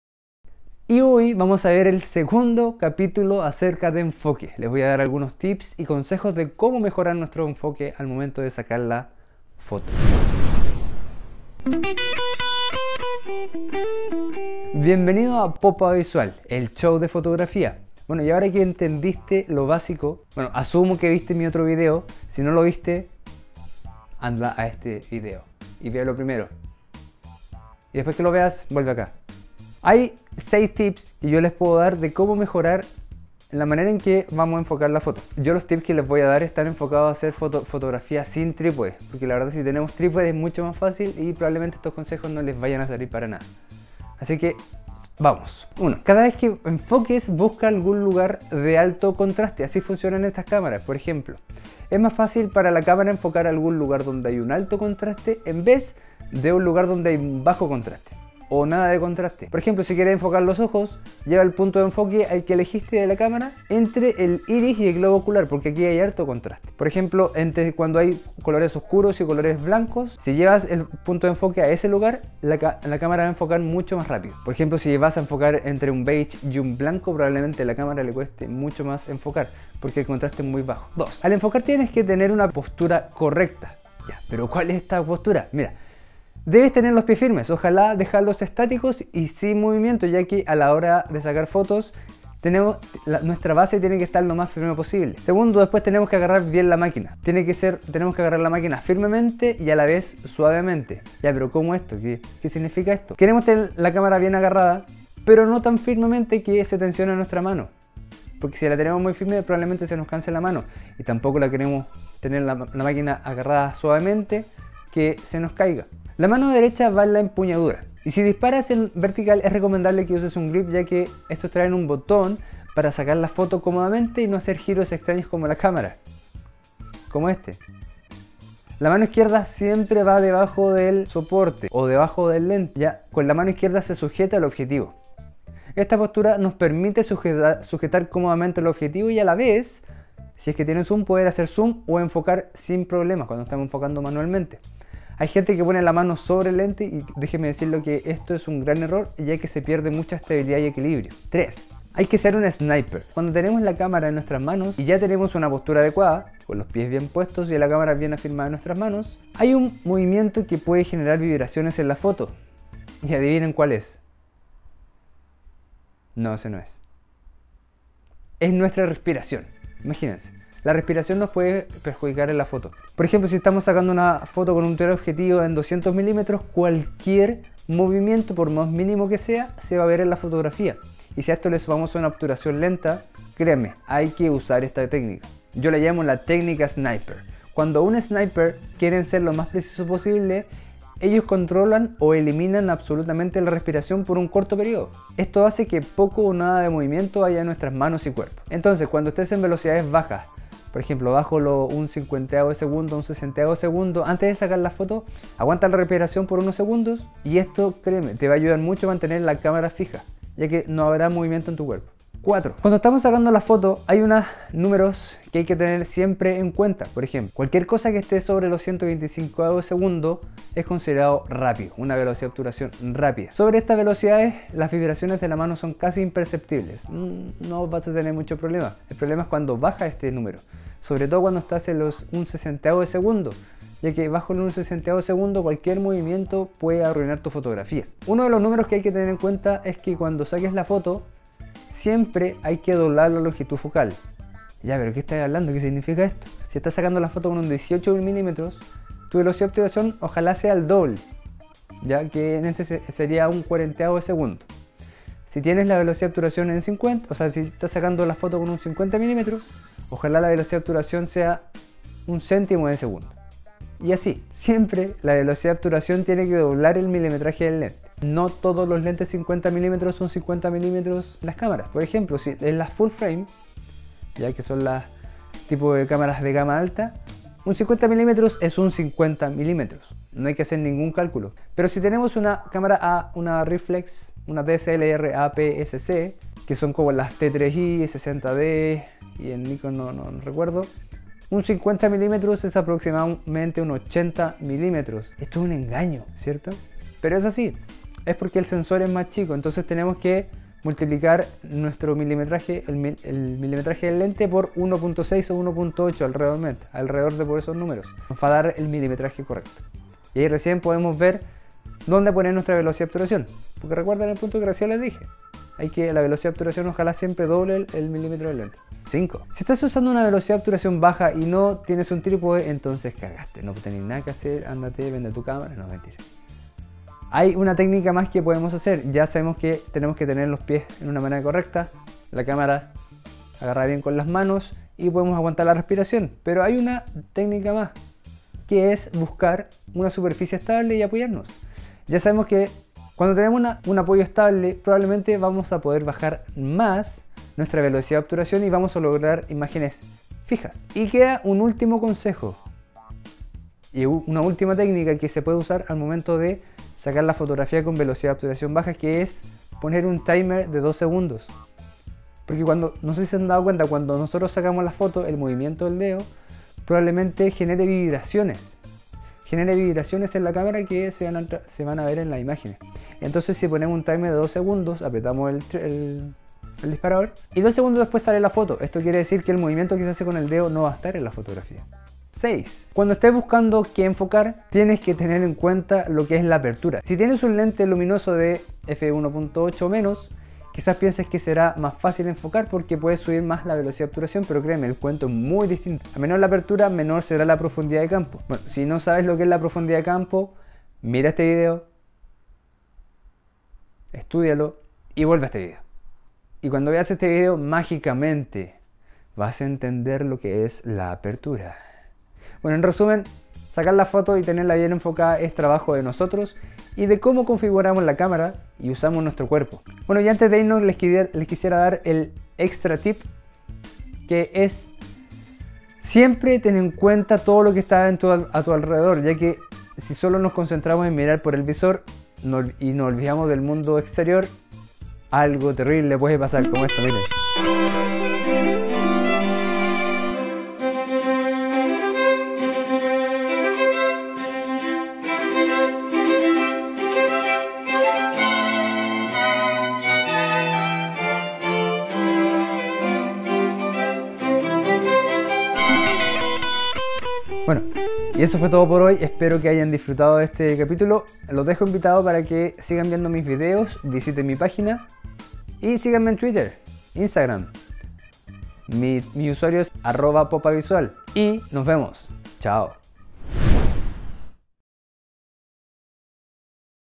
Popavisual - 6 consejos para enfocar mejor (Enfocate- cap2)
En el primer capitulo revise un poco la teoría en la cámara de como usar los distintas configuraciones para enfocar.

En este capitulo doy 6 consejos y tips de como mejorar el enfoque al momento de sacar la foto, que postura usar, como agarrar la cámara y hasta como respirar.